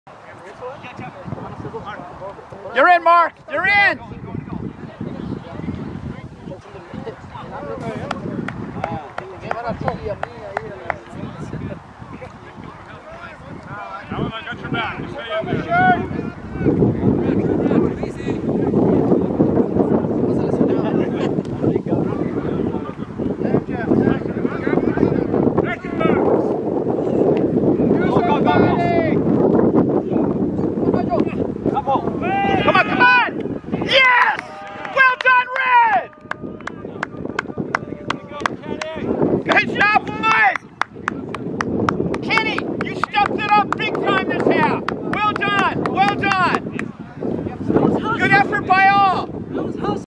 Kenny's Awesome Soccer Goal
The Masters Championship between Spartans and Rovers, December 2021
Videotaped by Patty Mooney, video producer at San Diego Video Production company Crystal Pyramid Productions